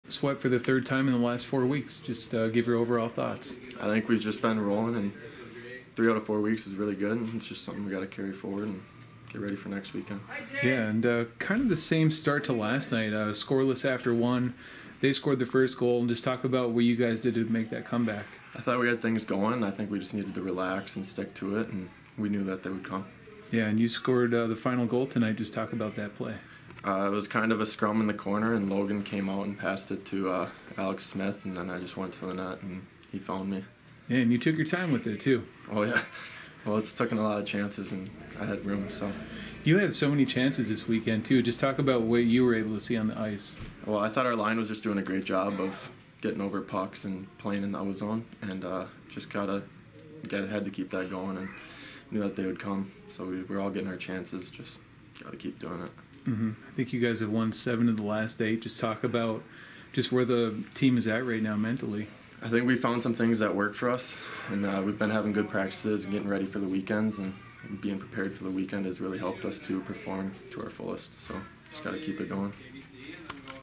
Alec Broetzman after Tech swept UAA
Michigan Tech sophomore forward Alec Broetzman talks about the 4-1 win and sweep of Alaska Anchorage. Broetzman scored the fourth goal for the Huskies.